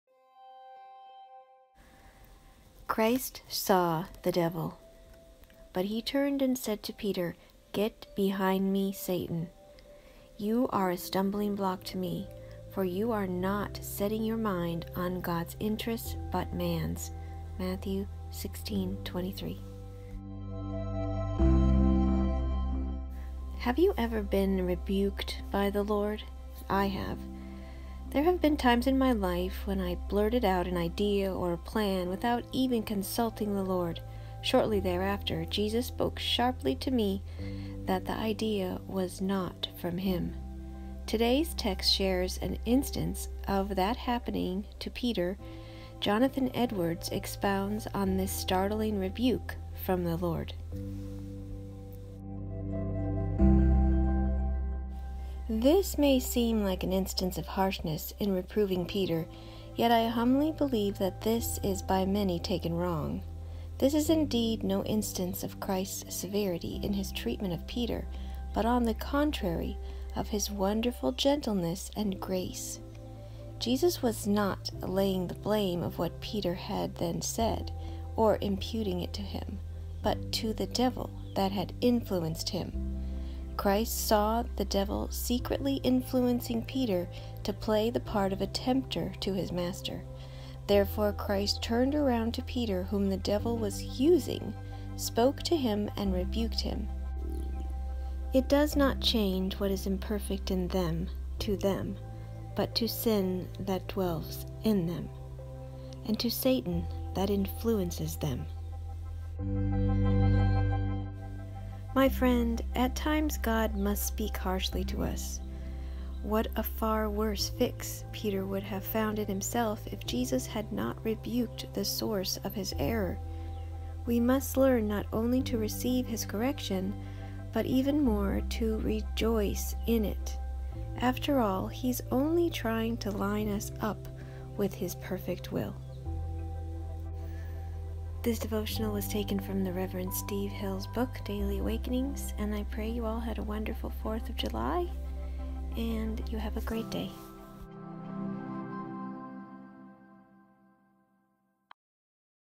July 5-Christ Saw the Devil
This devotional was taken from the Reverend Steve Hills book daily awakenings for the date of July 5. Used by permission.